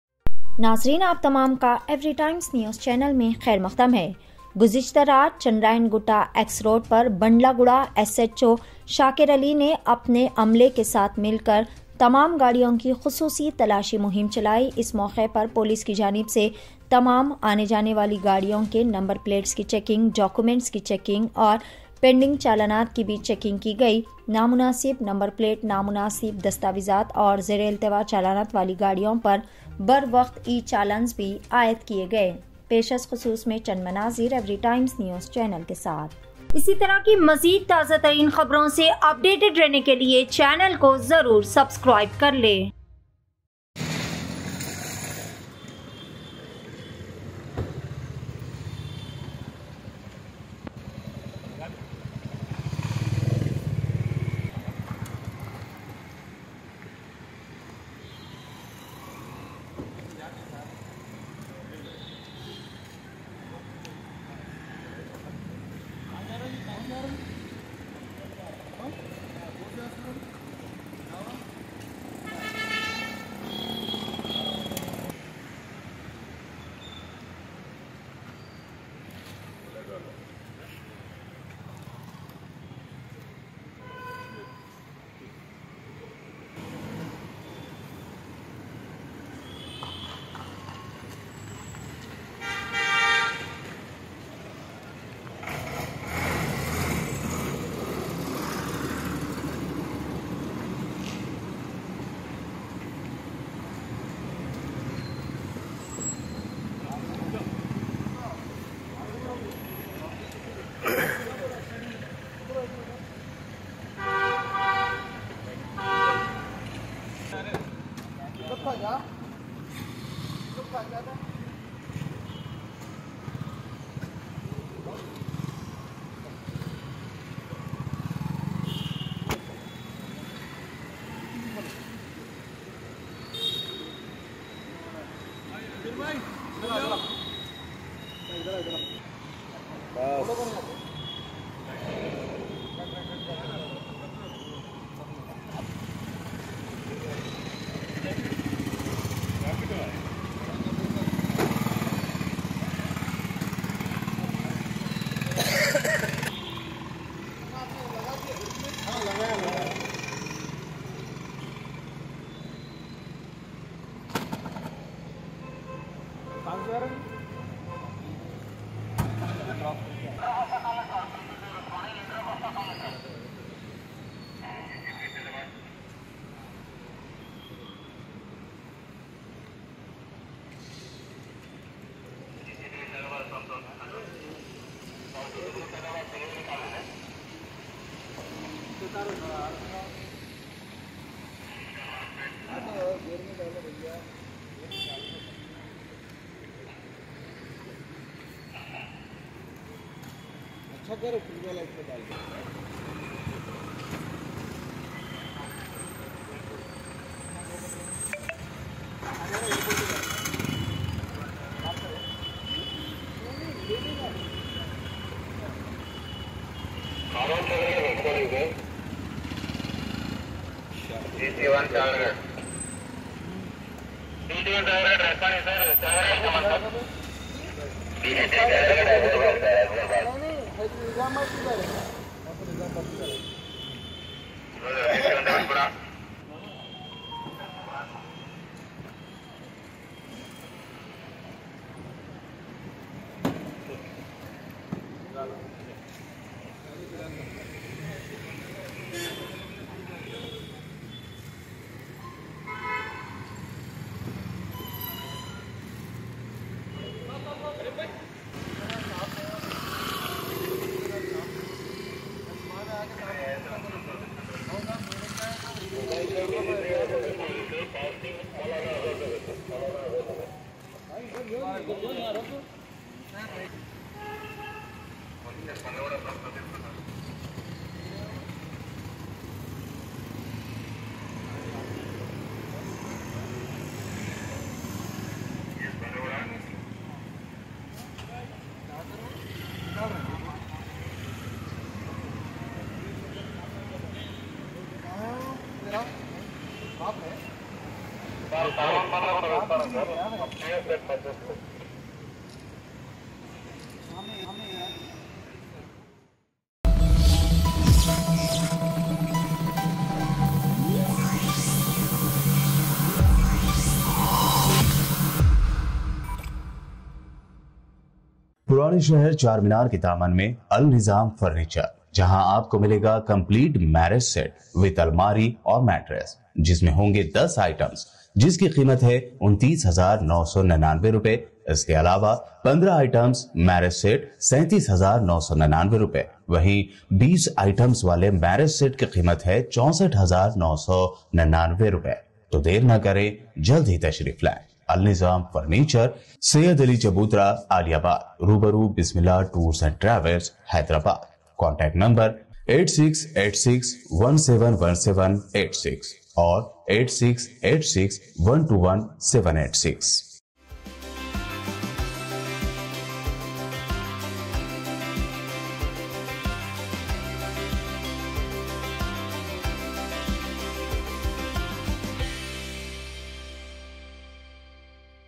Sardi Mein Bhi Police Apna Farz Nibhate Hue Aayee Nazar | Chandrayangutta Par Vihicle Checking...
Sardi Mein Bhi Police Apna Farz Nibhate Hue Aayee Nazar | Chandrayangutta Par Vihicle Checking |@Etnewshyderabad

FOR DETAILS:-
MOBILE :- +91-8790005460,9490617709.